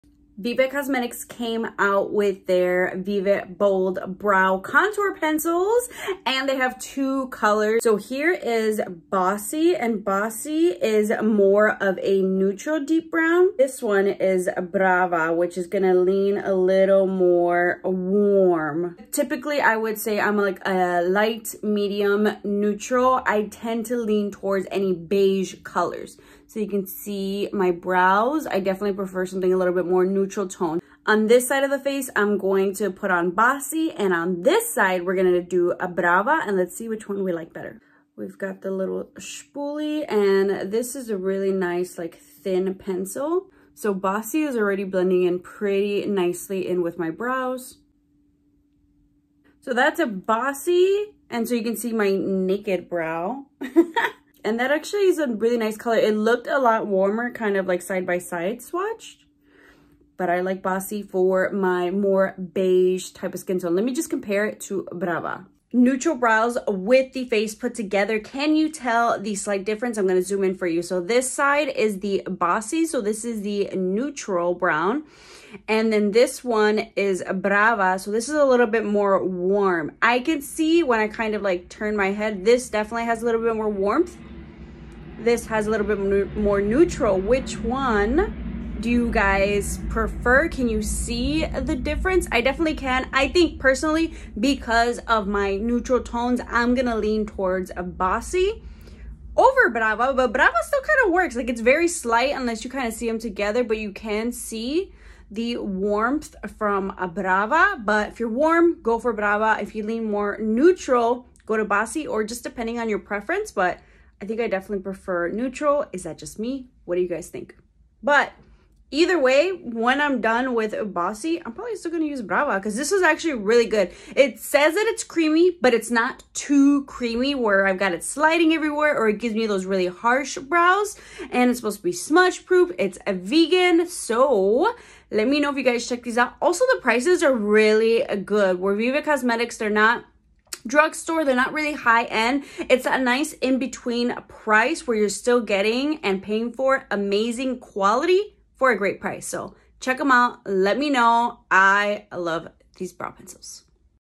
Vive Cosmetics Bold Brow (Bossy VS Brava)
Check out Vive Cosmetics New Bold Brow Pencils Swatched Side By Side